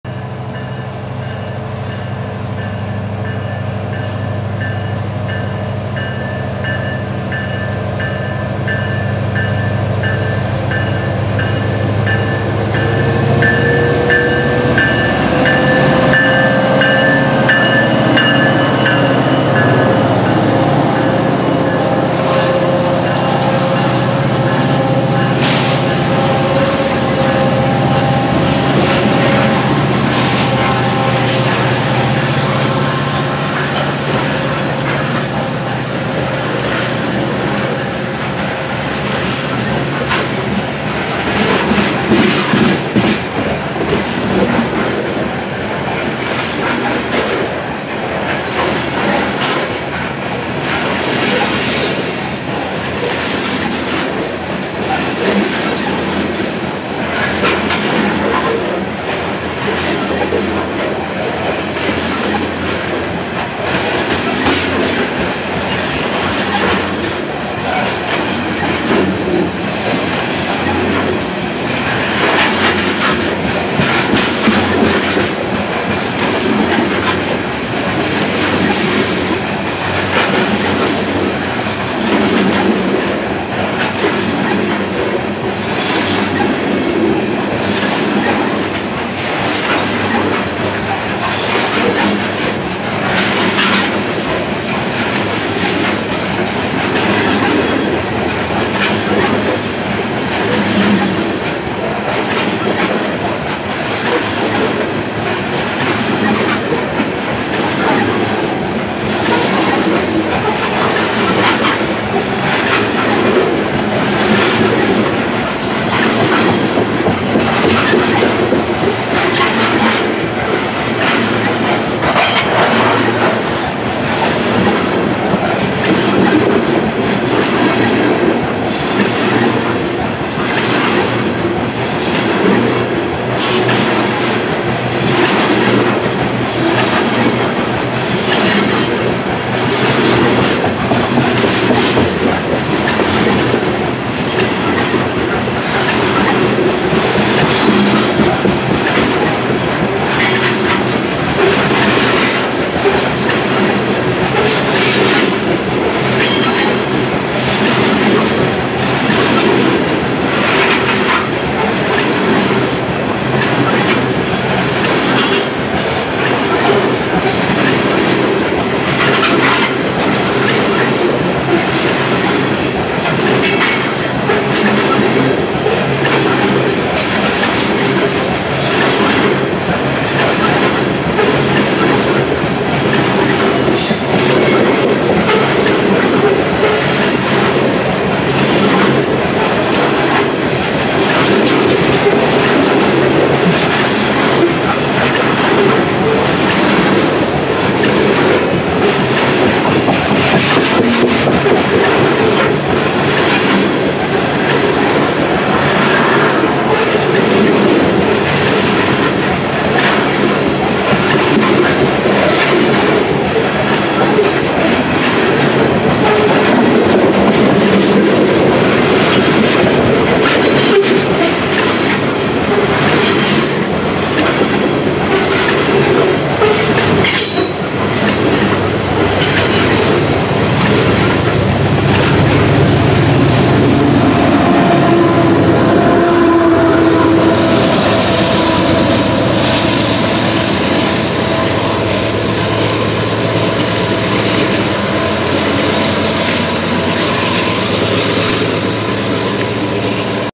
BNSF Coal Train Speeds Up On “The Racetrack” Thru the Rt 59 Metra Station - Aurora, IL
Standing on the platform of the Rt 59 Metra station, I saw this eastbound BNSF coal train slowly come around the curve to the west of the station as it exited the Eola Yard. As it approached the station it began to slowly pick up speed as it headed off to its destination in Chicago from the Black Thunder Mine in Wyoming.

Rt 59 sits at mile post 31.0 on the Chicago Subdivision.